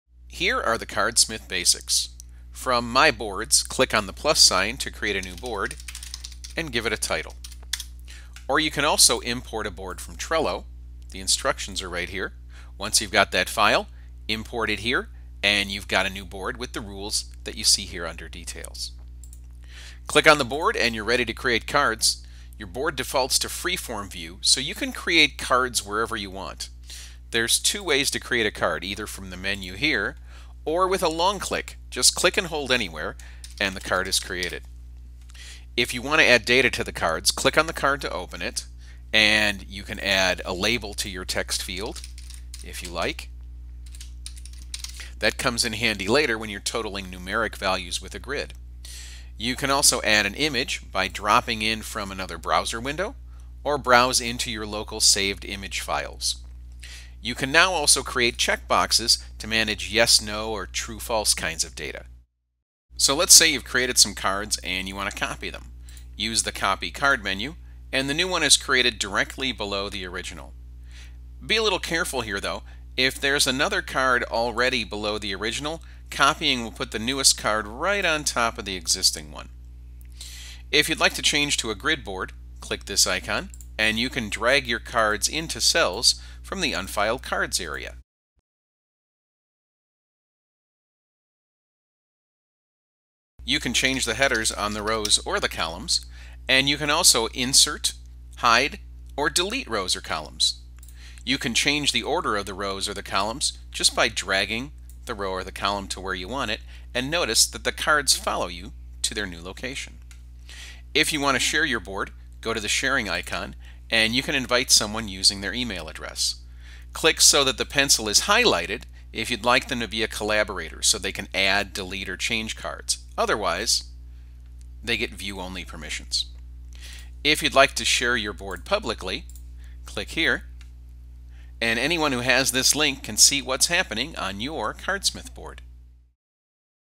How to Get Started with Cardsmith and Digital Sticky Notes
Your team already knows how to use Cardsmith - it’s just like unwrapping a new pad of sticky notes, only smarter. Create and manage your digital cards the same way you use stickies and a white board. Cardsmith lets you focus on your work, not on figuring out some new productivity tool. Learn the fundamentals of using Cardsmith, the web-based collaboration and project management tool. Learn how to create cards, get organized, and share your boards.